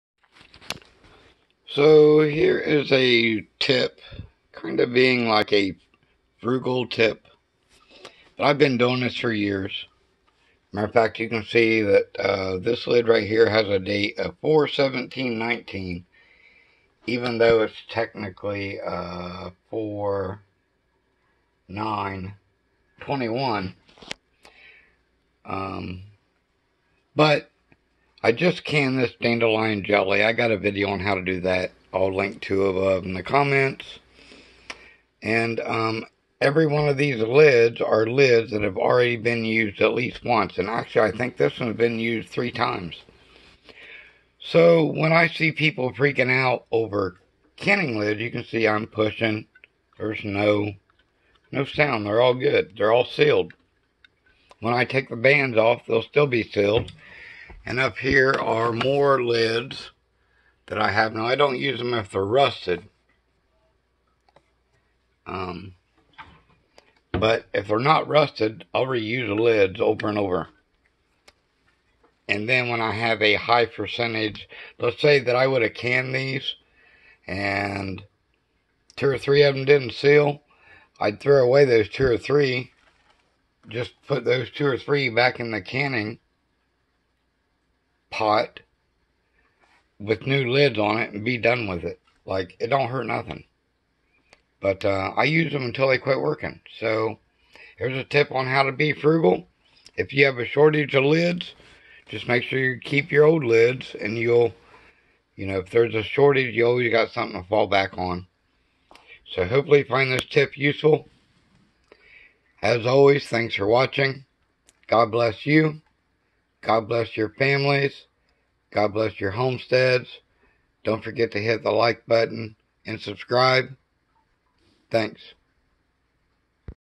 frugal on the homestead
just got done canning dandelion jelly, redbud will be next, usually followed by dehydrating morel mushrooms and then about mothers day the garden will be finished up with planting warmer crops.

I made this video because i am one if the most frugal, penny pinching people you could ever meet. With that being said, i share with you one if my most kept secrets I've been doing for years and will save you some money or maybe help you out during a crisis ... like so many people were facing in 2020 during the "covid years".